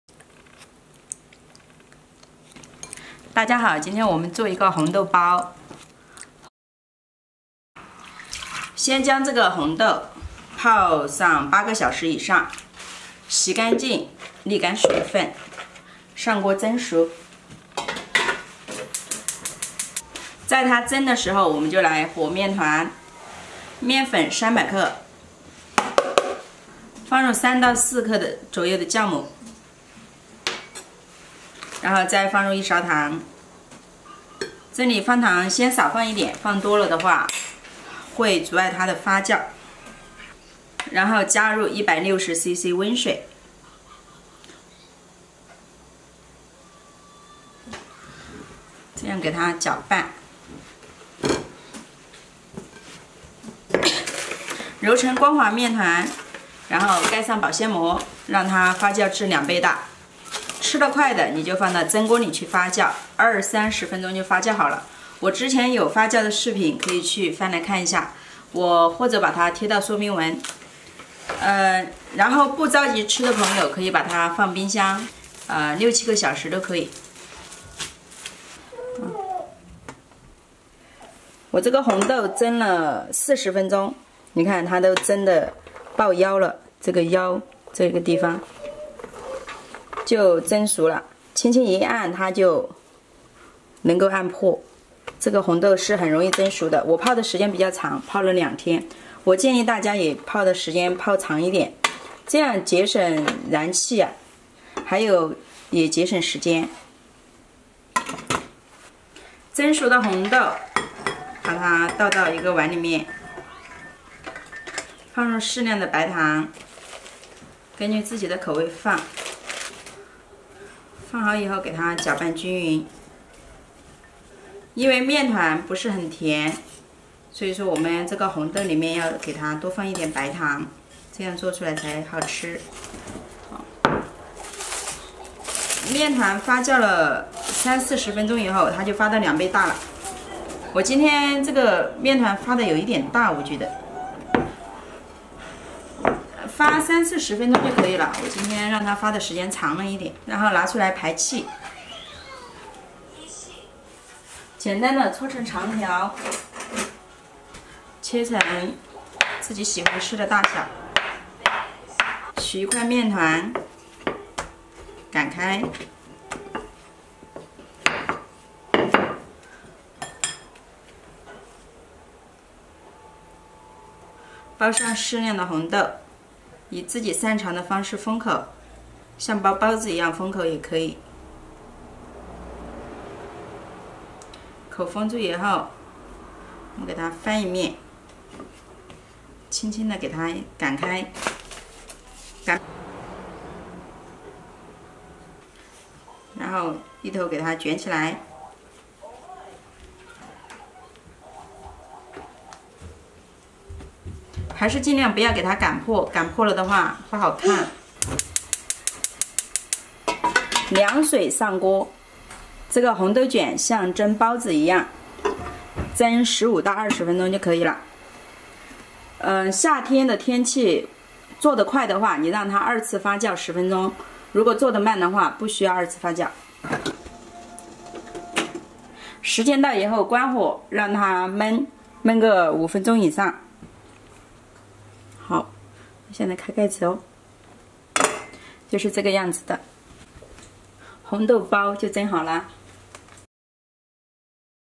红豆包，红豆控的理想吃法，多吃红豆排毒养颜喔
红豆包红豆卷分享给大家😊
欢迎大家订阅小芳的频道
面食，家常菜不定时更新，谢谢支持和鼓励。
本来今天还煎了三个红豆卷的，因为有事耽搁了一会回来就烤糊了。可惜了，其实像升级一样煎的红豆卷也应该非常好吃的。